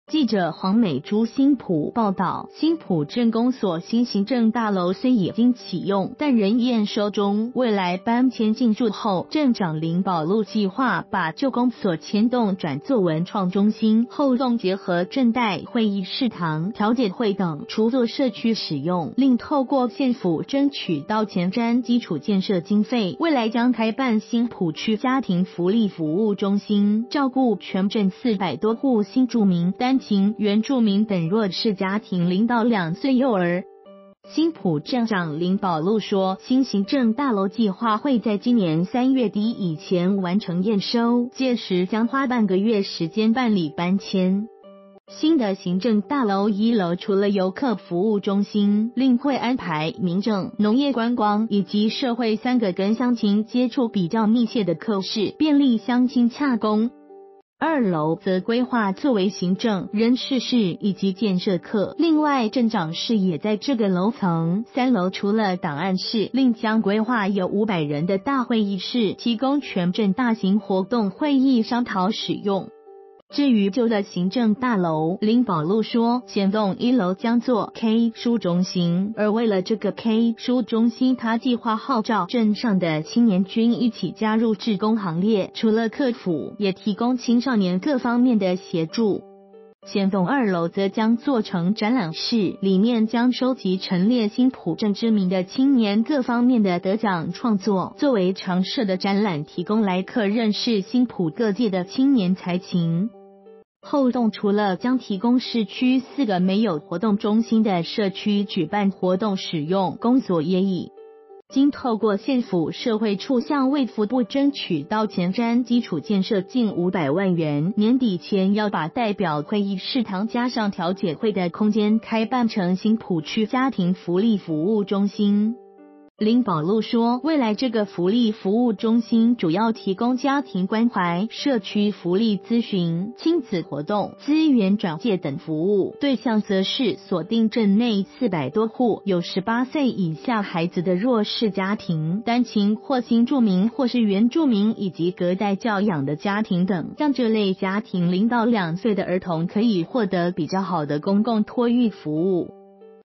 新埔鎮公所舊大樓 將轉做文創、家庭福利服務中心
〔記者黃美珠／新埔報導〕新埔鎮公所新行政大樓雖已經啟用，但仍驗收中，未來搬遷進駐後，鎮長林保祿計畫把舊公所前棟轉做文創中心，後棟結合鎮代會議事堂、調解會等，除做社區使用，另透過縣府爭取到前瞻基礎建設經費，未來將開辦「新埔區家庭福利服務中心」，照顧全鎮400多戶新住民、單親、原住民等弱勢家庭0到2歲幼兒。

發表評論評論如下。